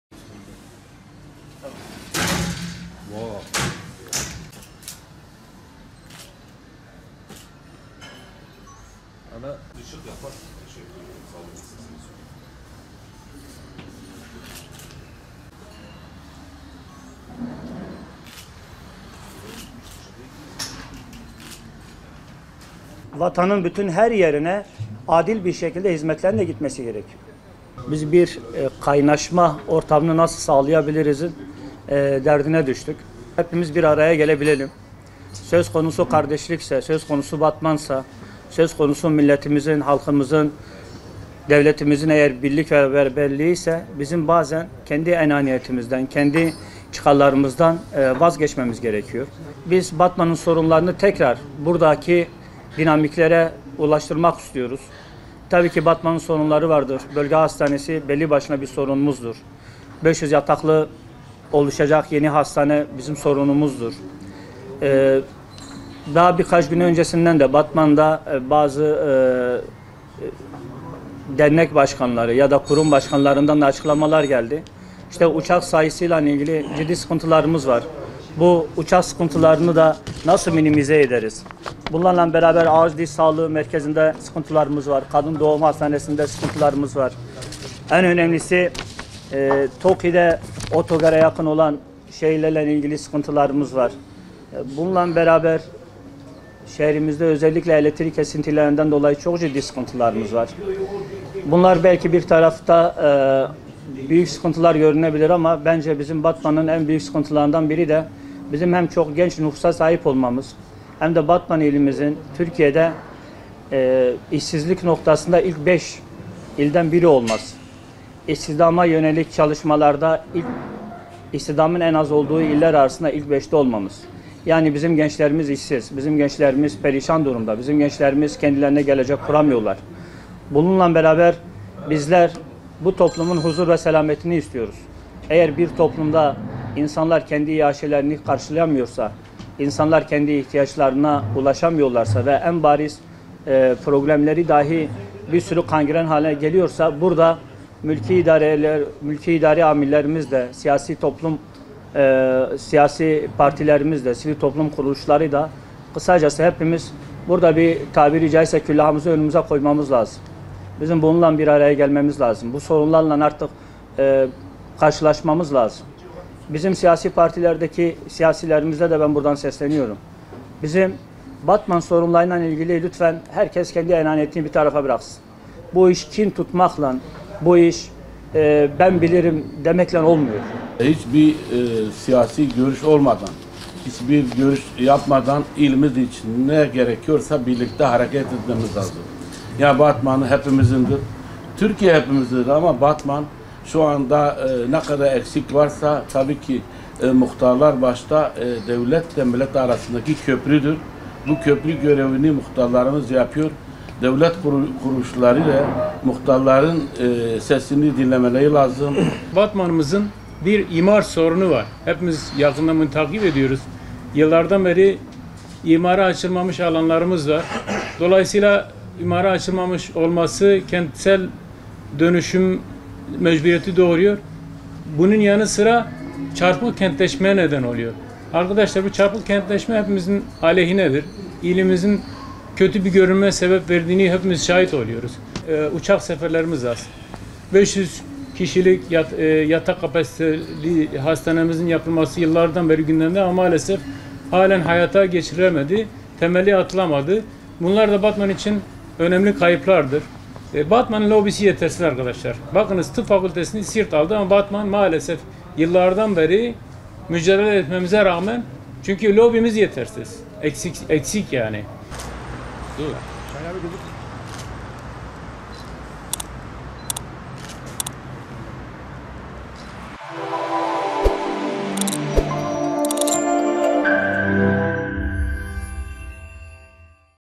Batman'da STK'lar, kentin sorunlarının çözümü için birlik çağrısında bulundu
İlin çözüm bekleyen birçok sorununun olduğuna dikkat çeken STK'lar, düzenledikleri basın toplantısında siyasiler başta olmak üzere herkesi bu kapsamda sorumluluk almaya çağırdı.

Değerli İzleyicilerimiz;

TÜRKÇE

KÜRTÇE-KURMANCI

ARAPÇA

İNGİLİZCE

FARSÇA

KÜRTÇE-SORANİ